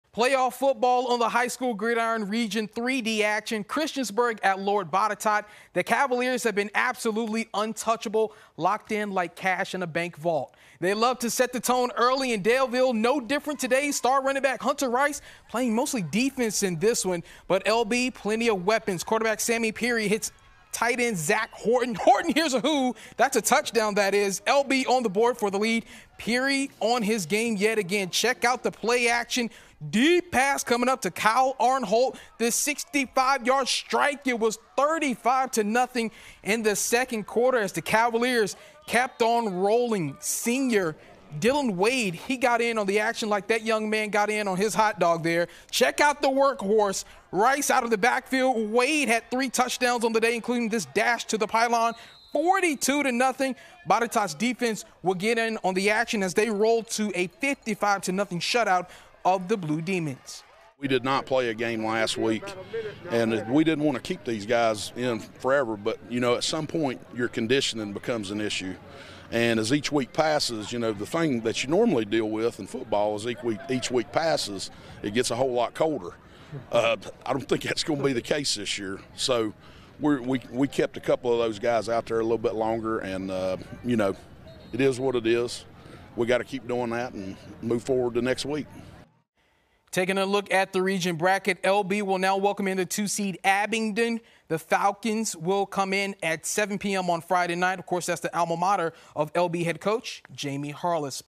Lord Botetourt takes down Christiansburg in Region 3D playoffs
Cavaliers beat the Blue Demons, 55-0